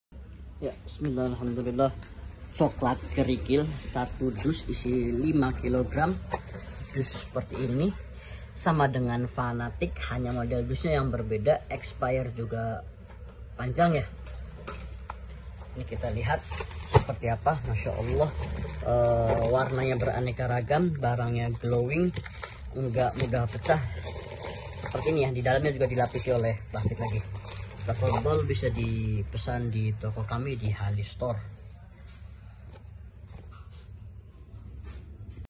COKELAT KERIKIL DUS ISI 5KG Cokelat kerikil 5kg Dus 5kg Coklat Krikil Cokelat Kerikil Cokelat Batu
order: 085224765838

COKELAT KERIKIL DUS CHOCOLATE STONE DRAGEES Merk Dus FANATIK, Kualitas & Rasa Sama dengan Merk Fanatik (COKELAT COMPOUND SUSU ANEKA WARNA) Kemasan Dus isi 5kg.

Stone Cokelat atau disebut Cokelat Kerikil / Cokelat Batu / Cokelat Arab / Cokelat Turki TERSEDIA KEMASAN Dus ASLI ISI 5KG.

Cokelat Kerikil merk Fanatik.

Deskripsi Lengkap Stone Chocolate (Cokelat Kerikil):
- kemasan: Dus isi 5kg
- Rasa: cokelat
- Tekstur: Lebih Soft & lumer di mulut (tidak keras)
- Warna: Aneka Warna
- Expire: 03/2024.

*Kemasan Asli dari Importir ada label revisi putih & kemasan dalam dus plastik terbuka.

Barang JAMINAN FRESH, BAGUS (Tidak Lengket di Tangan) dan Tidak Hancur.

Silahkan di order.